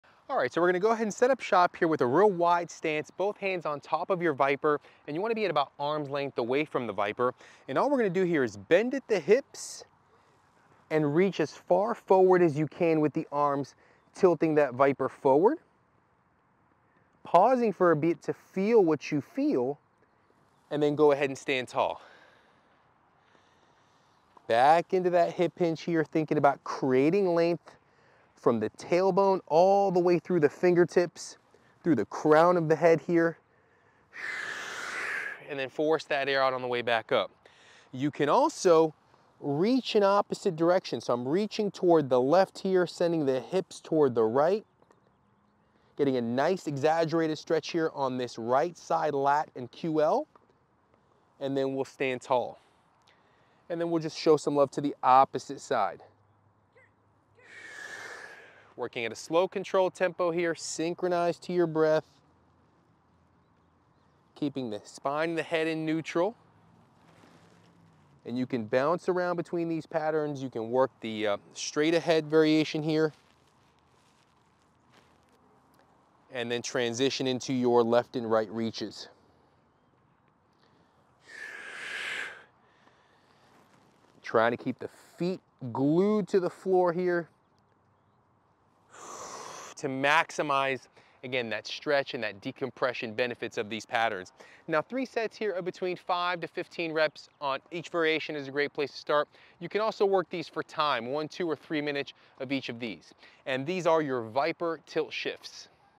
ViPR Tilt Shift
The ViPR Pro is excellent for mobility training, promoting increased flexibility, joint range of motion, and dynamic stability. Its versatile design supports natural, multi-directional movements that enhance overall body control and fluidity, making it ideal for improving functional mobility and reducing injury risk.